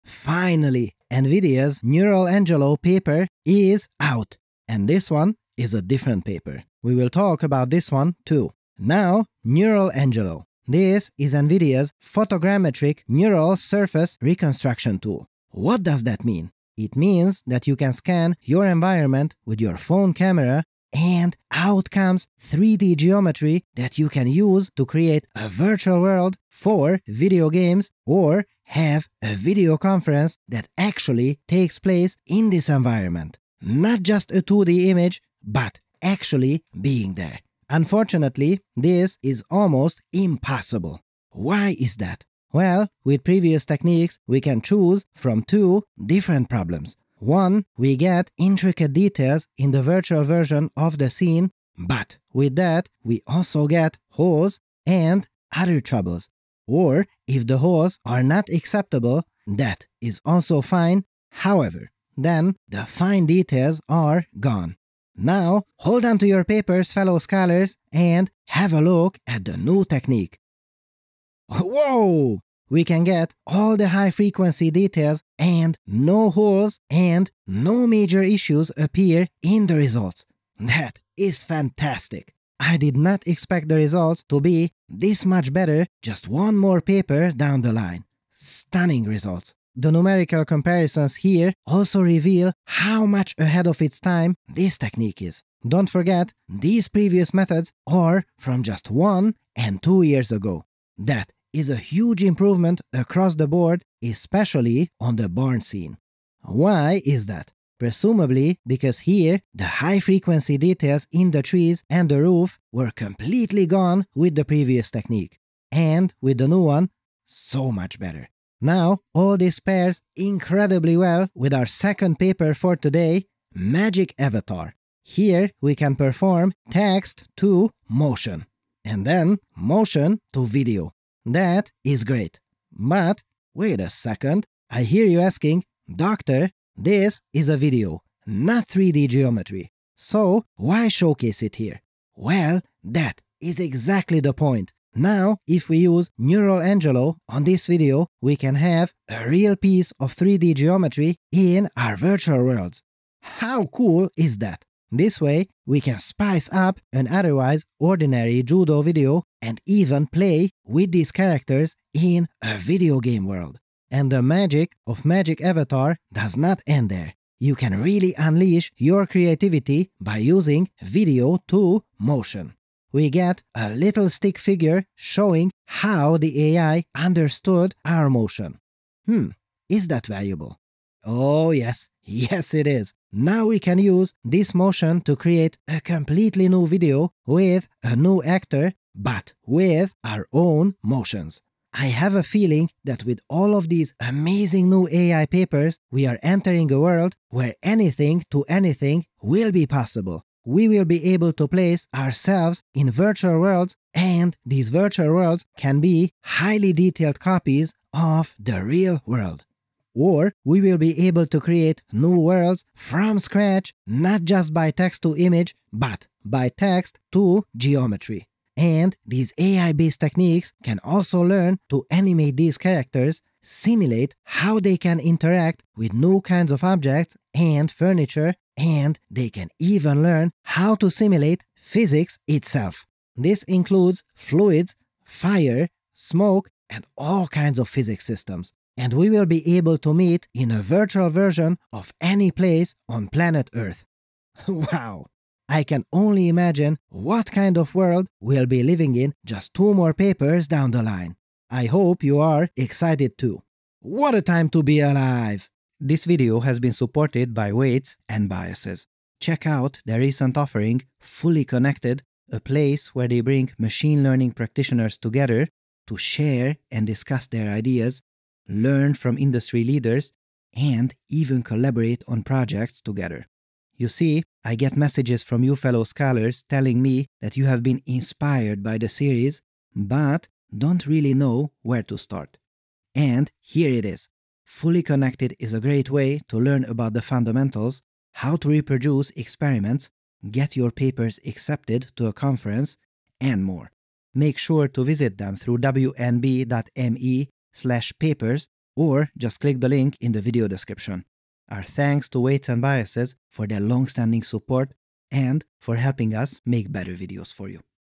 NVIDIA’s Neuralangelo AI: Gaming Anywhere on Earth!
📝 The paper "Neuralangelo: High-Fidelity Neural Surface Reconstruction " and "Magicavatar: Multimodal Avatar Generation and Animation" are available here: